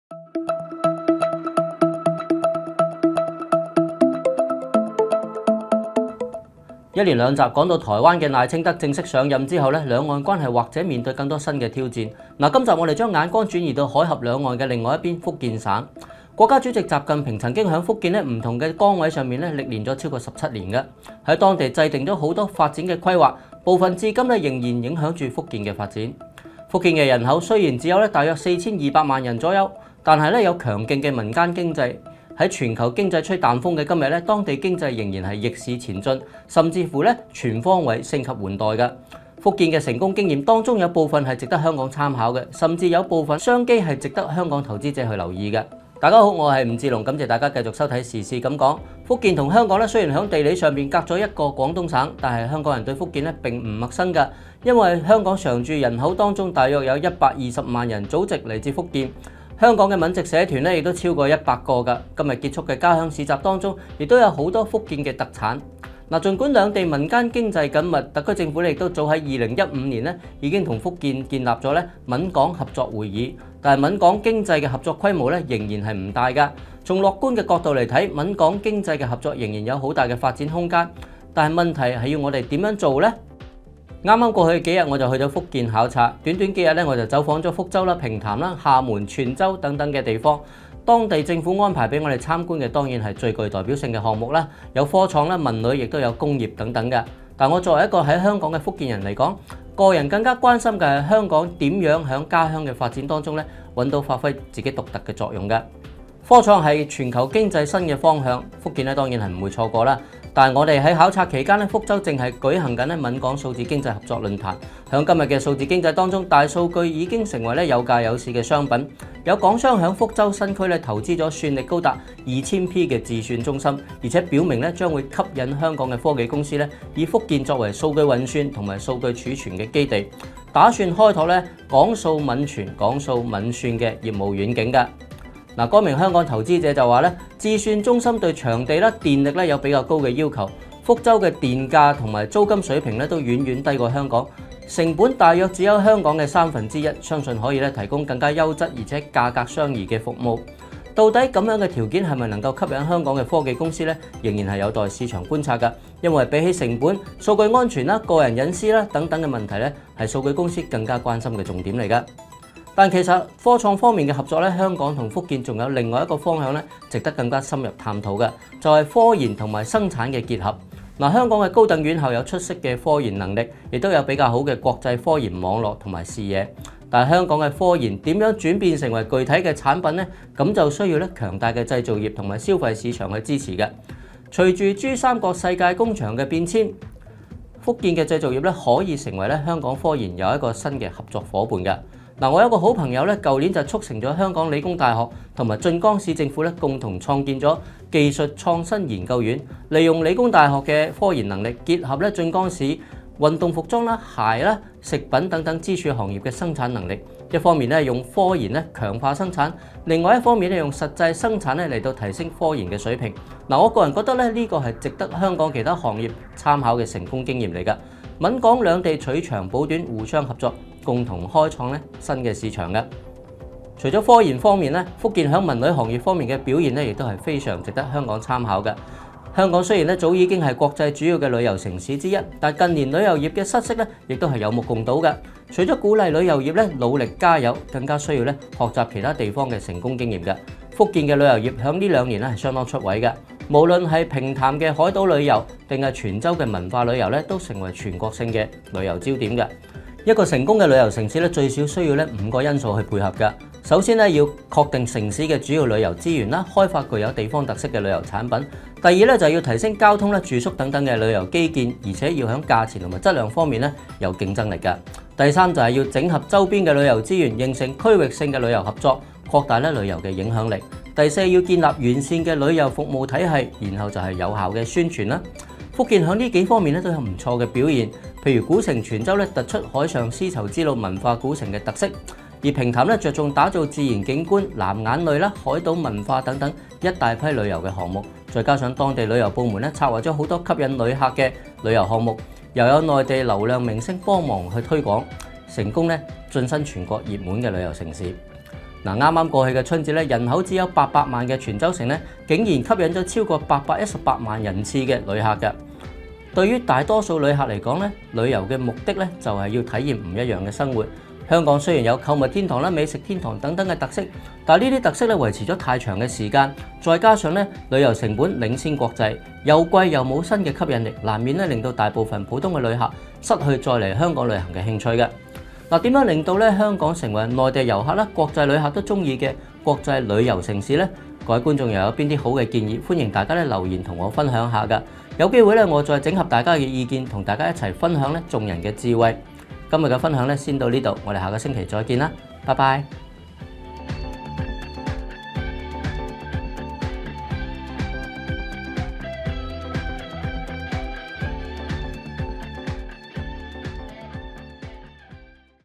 【時事敢講】（EP136） 閩港合作科技與旅遊先行 吳志隆：福建成功經驗值得香港借鏡
福建的人口雖然只有大約4,200萬人，但有強勁的民間經濟，在全球經濟吹淡風的今日，當地經濟仍然逆市前進，甚至全方位升級換代。福建的成功經驗當中有部分值得香港參考，甚至有部分商機值得香港投資者留意。